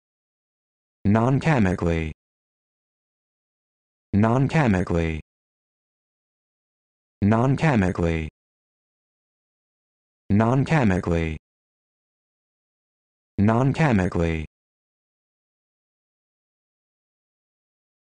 How to Pronounce Nonchemically
This video shows you how to pronounce Nonchemically